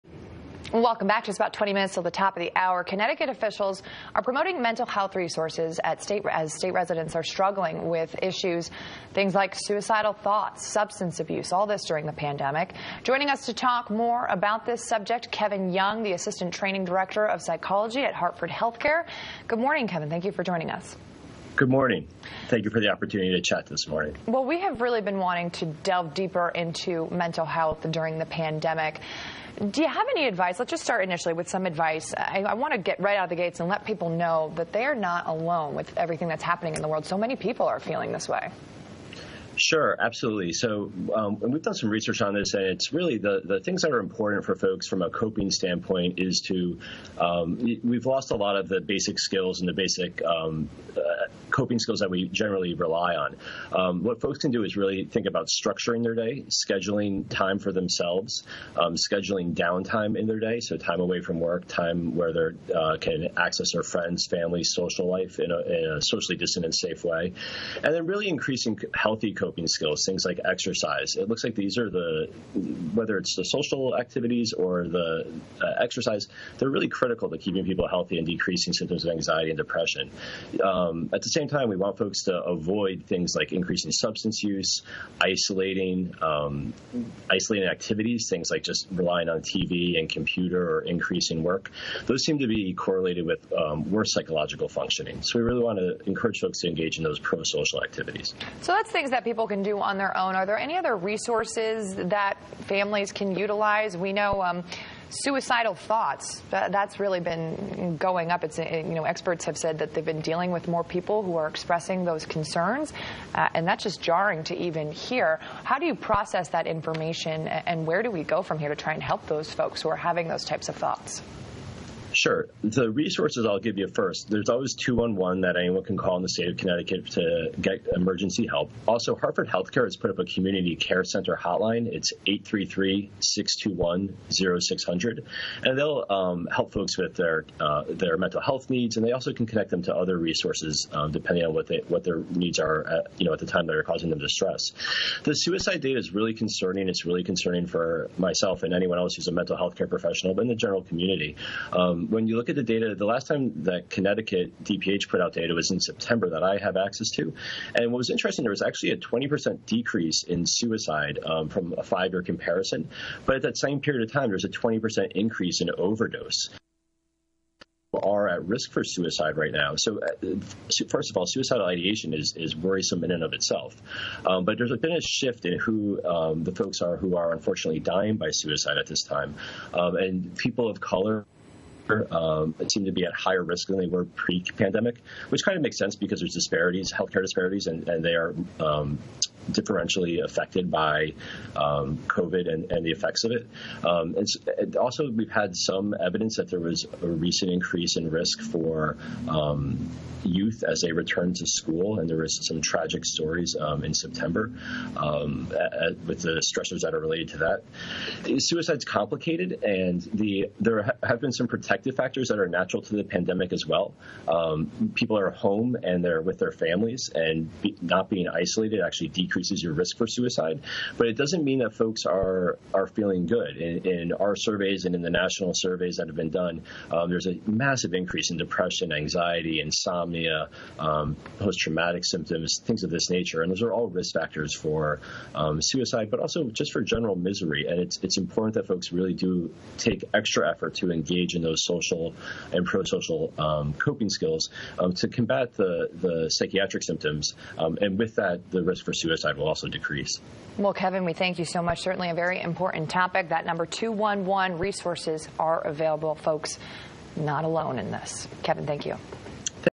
Mental Health Concerns During Pandemic
Kevin Young, Assistant Training Director of Psychology at Hartford HealthCare, talks about mental health concerns during the pandemic, how he works to connect people with resources and make them realize they are not alone in these struggles.